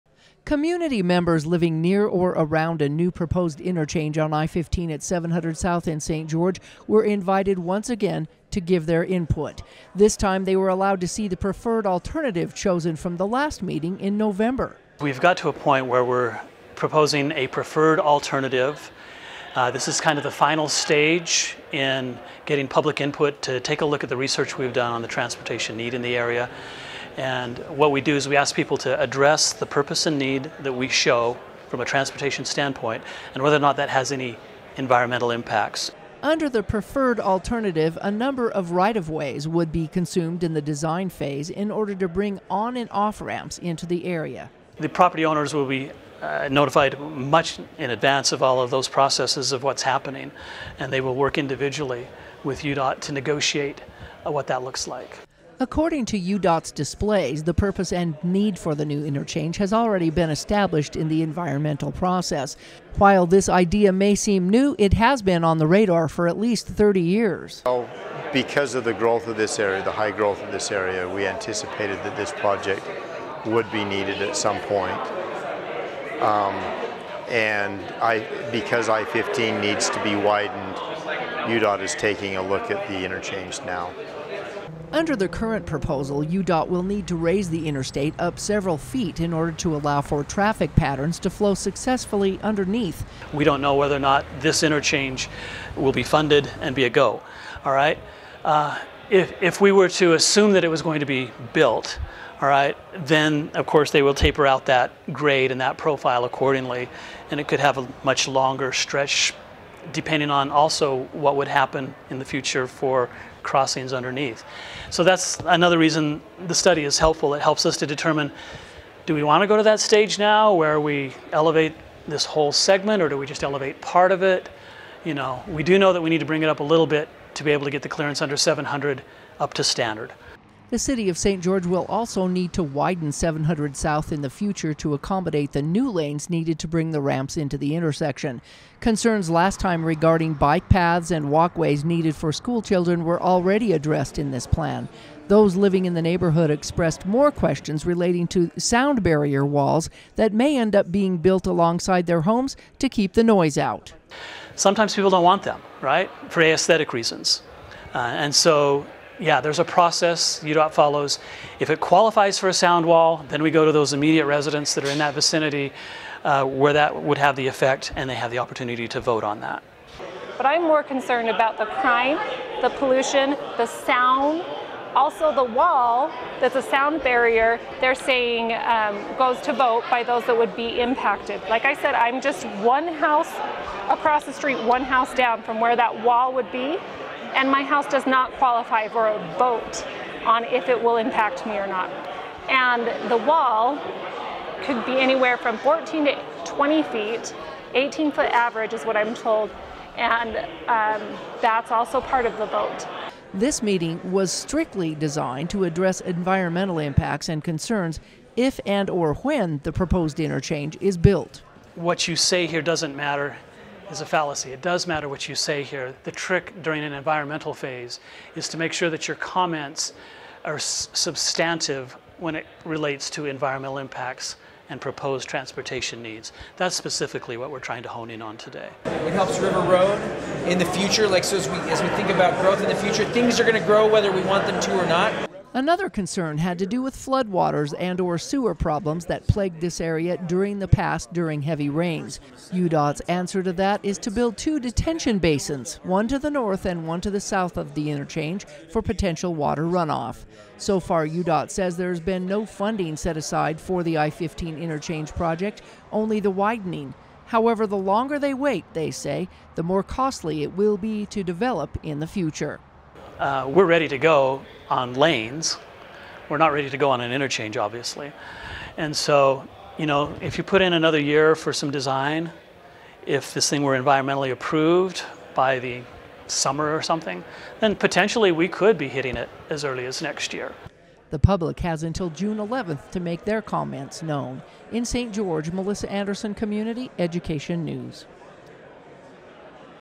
I 15 700 South Interchange Environmental Study
UDOT welcomes environmental feedback on 1-15 700 South Interchange

ST. GEORGE, Utah (5/17/2023) –  The Utah Department of Transportation held another in-person meeting regarding a proposed interchange at 700 South in St. George.
This time UDOT is looking for feedback on environmental impacts the proposed changes might have on community members living in the area.
While the study is still underway, UDOT spokesman Kevin Kitchen says the plan to widen the interstate in that location is still projected in 2024.
The public has until June 11 to submit their comments on the environmental impacts of the proposed interchange.